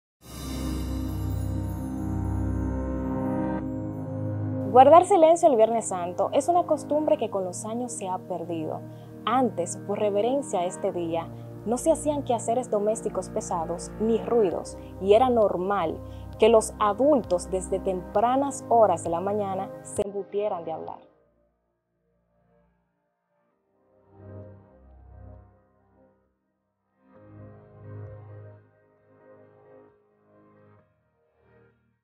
Guardar silencio en Viernes Santo
Guardar silencio en el Viernes Santo es una tradición que se ha perdido. Antes no se hacían quehaceres domésticos ni se podía emitir algún tipo de ruido.